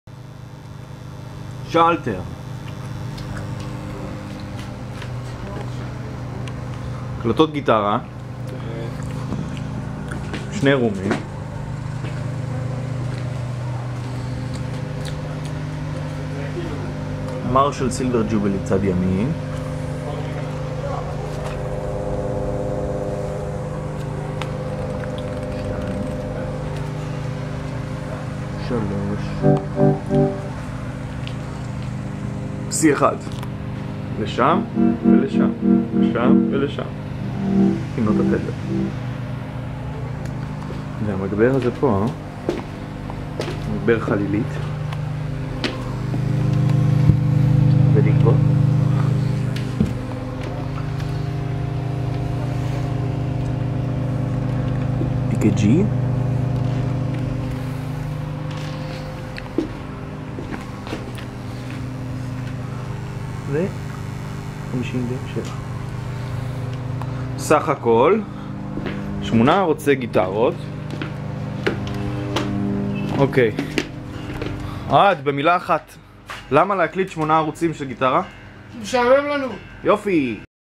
shiny space bar - guitar recording setup
guitar  recording setup
shiny space bar - alternative project by
maor wiesel - guitars
ziv ungar - drums
ohad shahar - bass
saar liven - vocal
recorded at "shalter studio" tel aviv 2013